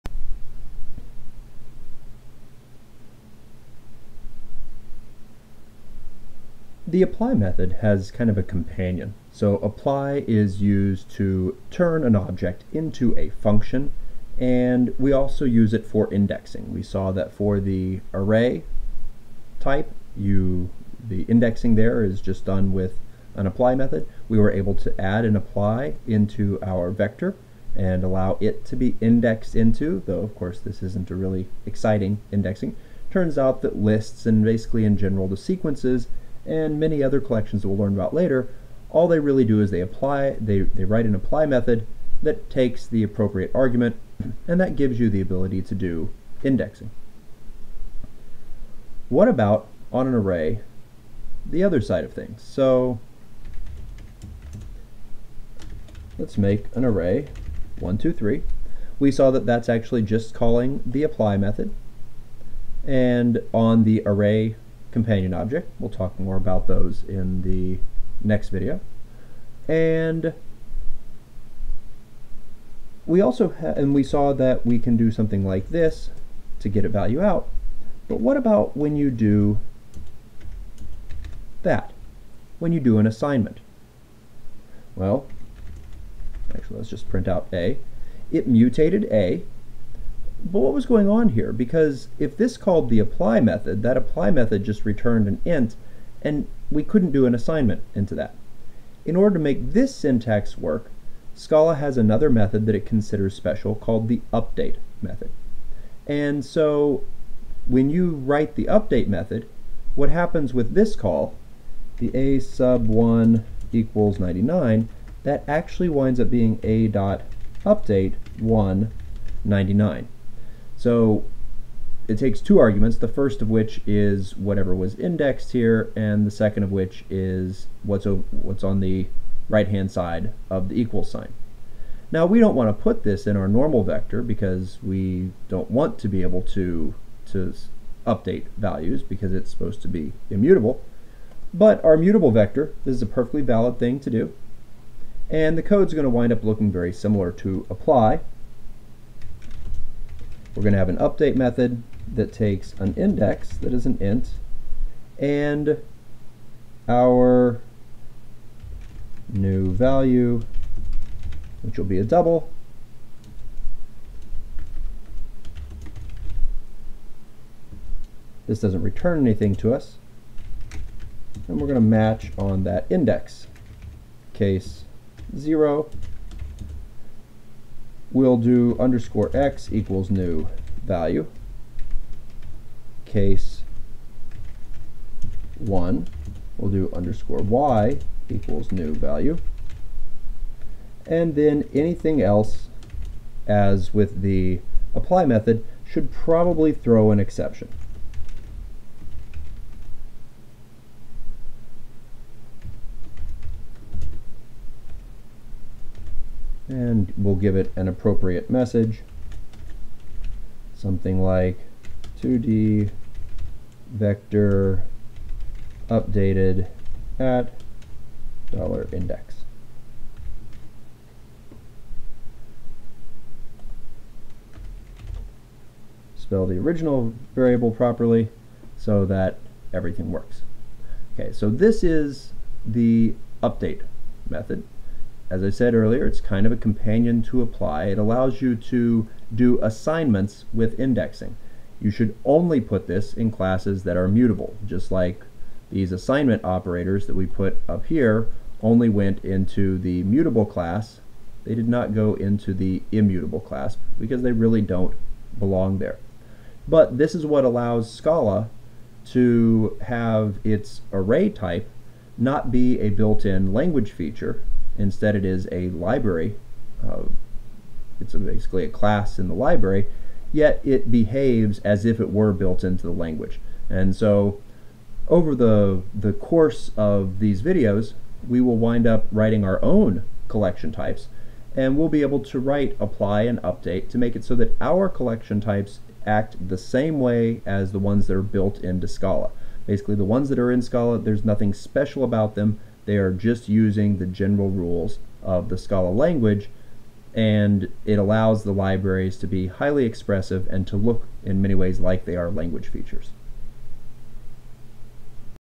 The update Method (in Scala)
This video introduces the update method in Scala for adding the ability to do assignments for indexed values.